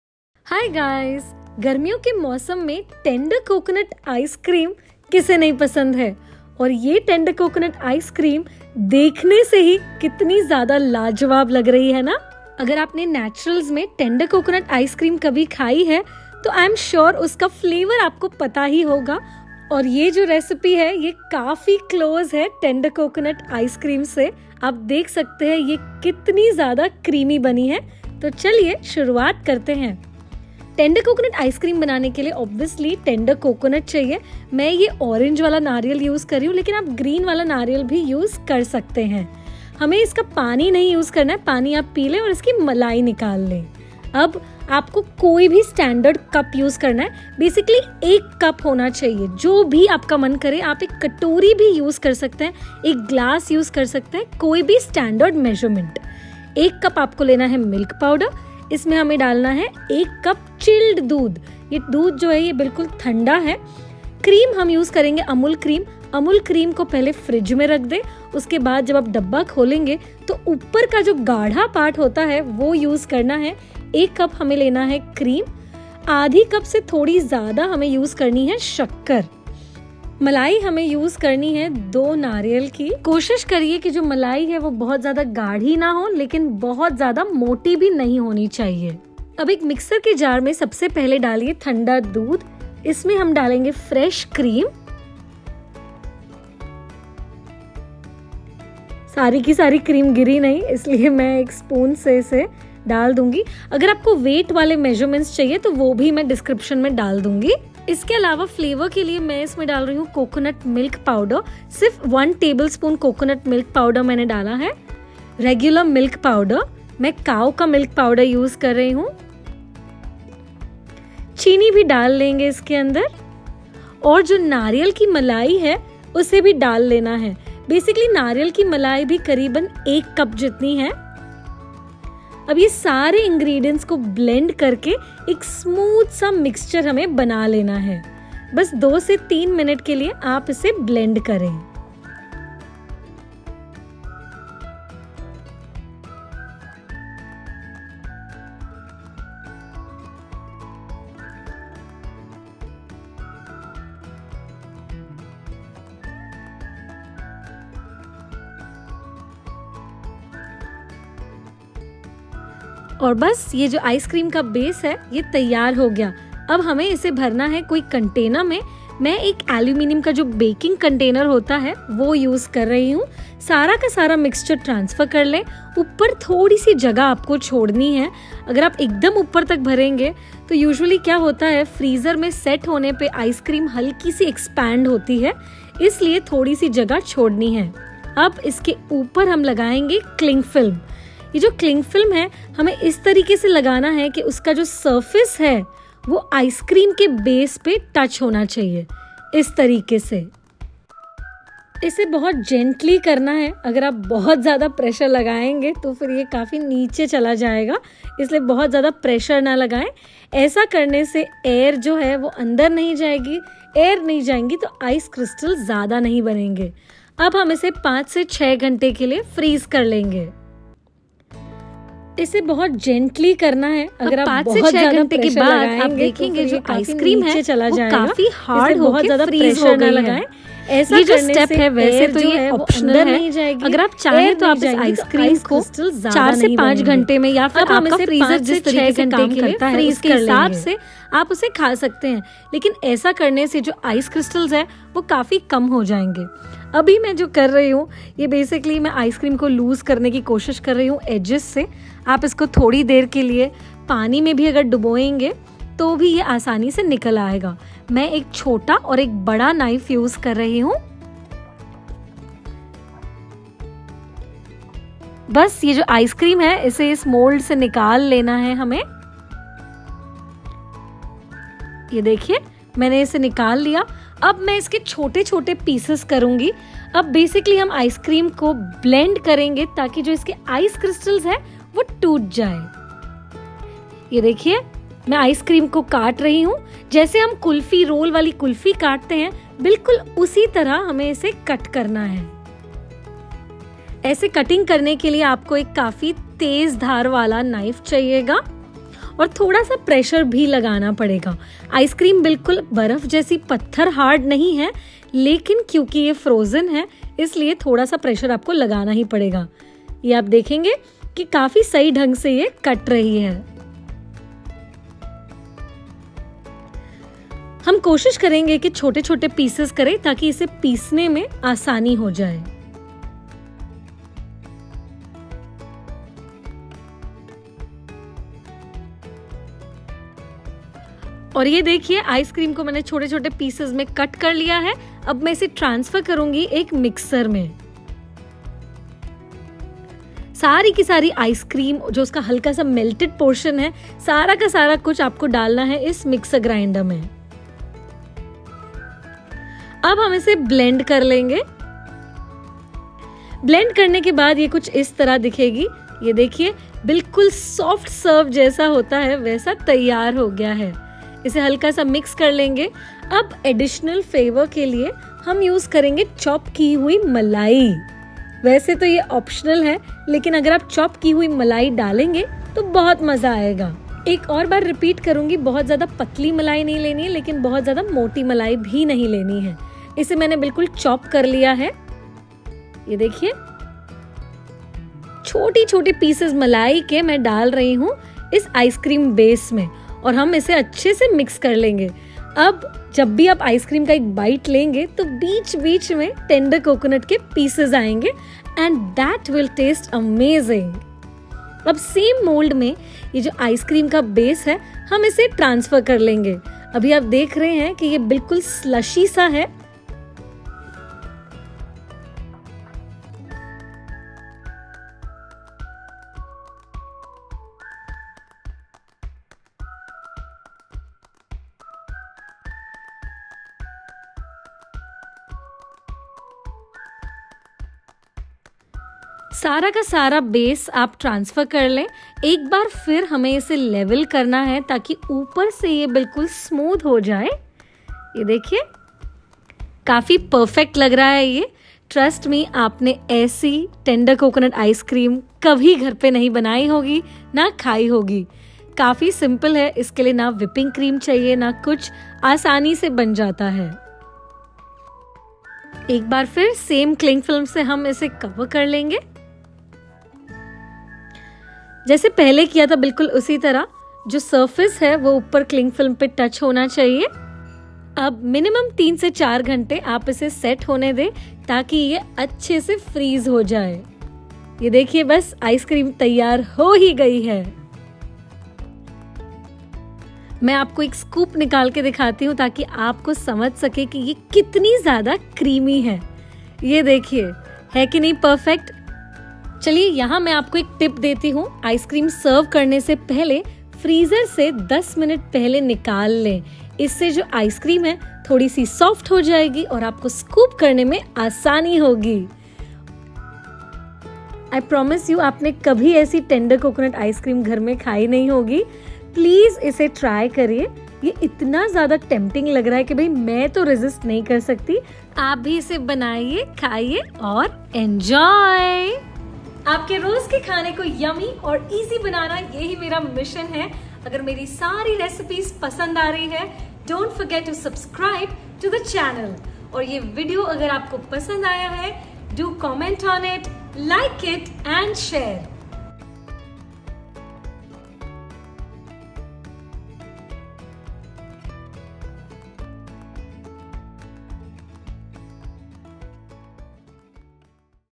NATURAL TENDER COCONUT ICE CREAM Recipe - No Whipping Cream Condensed milk. No Ice Cream Machine.
If you have tasted the original tender coconut ice cream. You will not be able to tell the difference. It is SOO GOOD !

The is a slight glitch at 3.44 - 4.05 Sorry for that !! (You can just forward those 15 seconds) Love you for understanding. Chotu mistake & Excitement ka combination

Ingredients-
Tender coconut malai (from 2 coconut yields, divided)
1 cup milk powder
1 cup chilled milk
1 cup cream( top thick part of Amul Fresh Cream after refrigerating it)
A little more than 1/2 cup sugar
1 tbsp coconut milk powder

Method-
Mix together all the ingredients along with 1/2 of the tender coconut malai in a blender jar and pulse until smooth.
Transfer into a container and cover with a cling wrap, while touching it on the surface of the icecream.
Freeze for 5-6 hrs.
Post 5-6 hrs, loosen it out, cut into small pieces and blend once again. This is to ensure the removal of all the ice crystals and make your icecream super creamy.
Chop the remaining malai, add into the blended mixture and transfer it all back into the freezing container. Cover it up with the cling wrap and freeze for another 3-4 hrs.
Remove, scoop out and enjoy!!

Vishesh Tippani-
Ensure to leave some space on the top of the freezing container.
While, putting the cling wrap its surface must touch the icecream base. This prevents any air incorporation and formation of ice crystals.
While making a homemade icecream, always freeze it 7 to 8 hours and then blend it using a high speed mixer and freeze for another 5-6 hours.
This helps in breaking the ice crystals formed, resulting in tastier and creamier end results.
Don’t put too much pressure on the cling film. You can directly eat ice cream after freezing but next step will give good results.